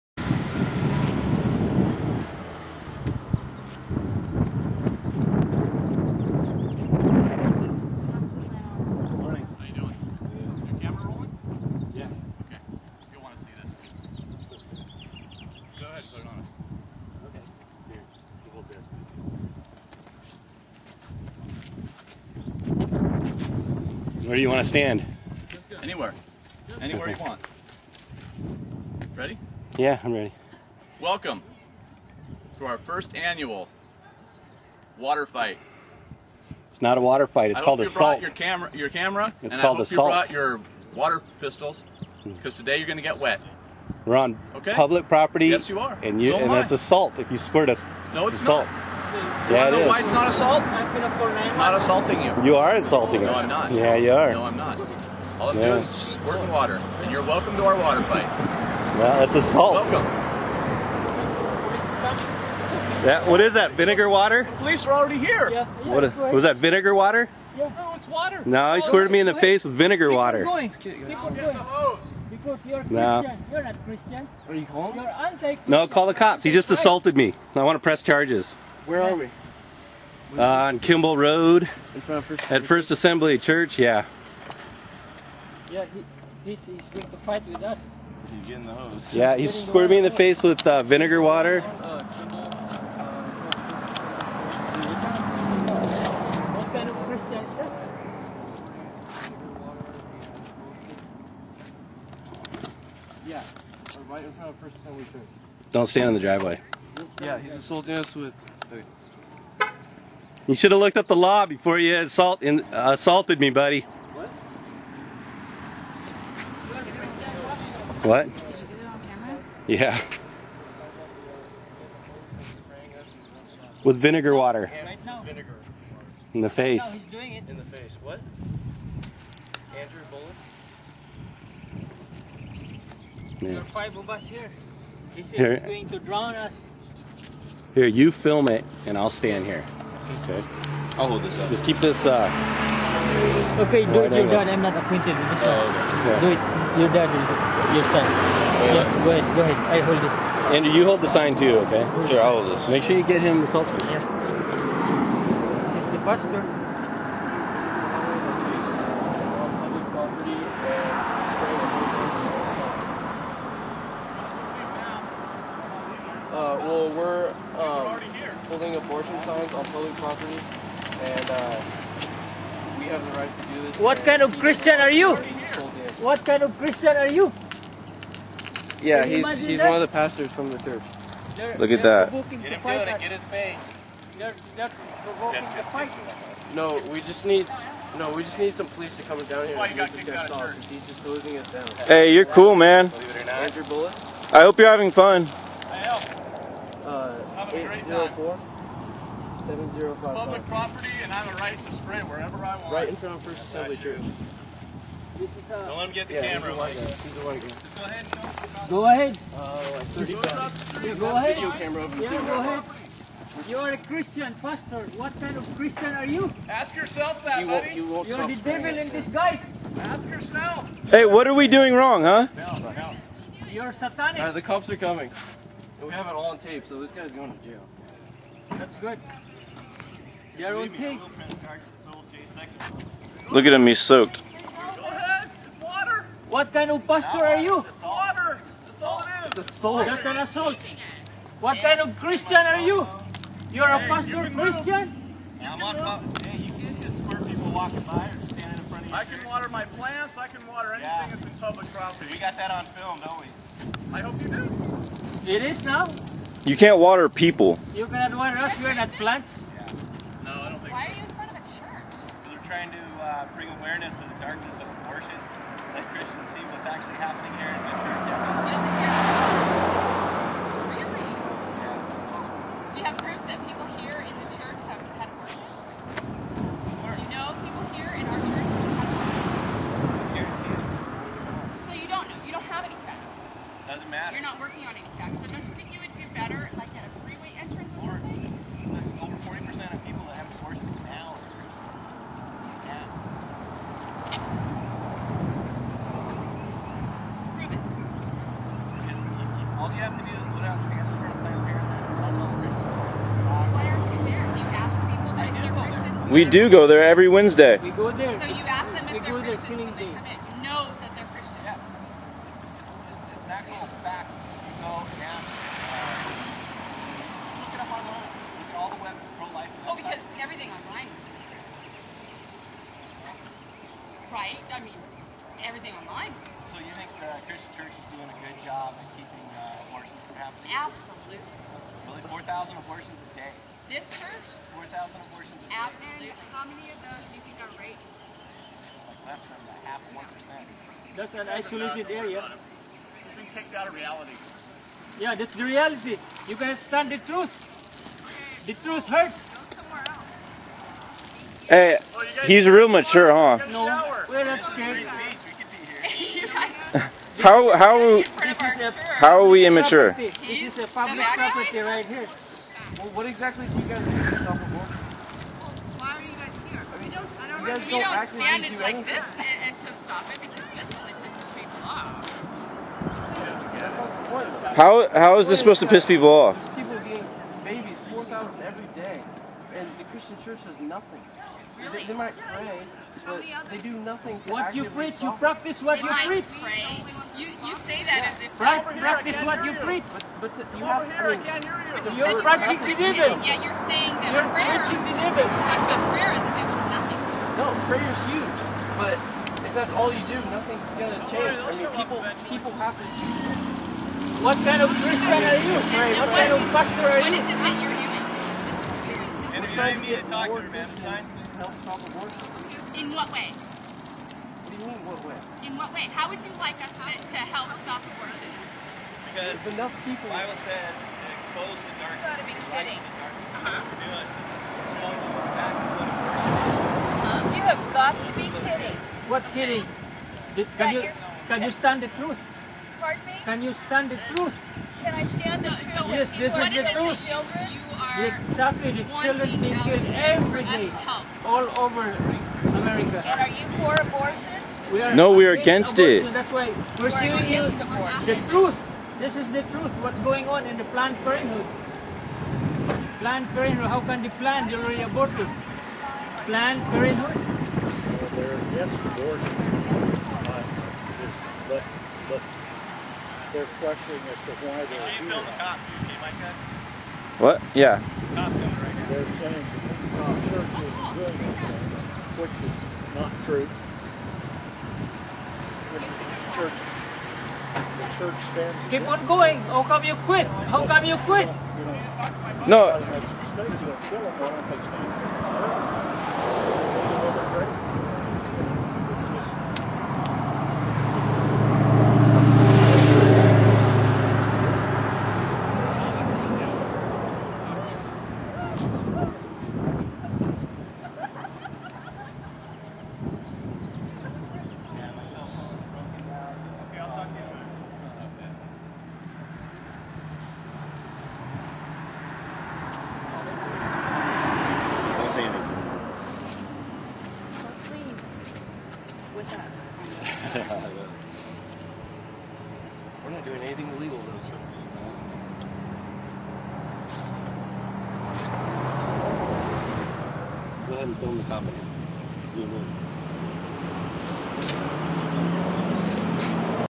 Jon Hixson
Jon Hixson a offduty cop sprays down protester out side of his church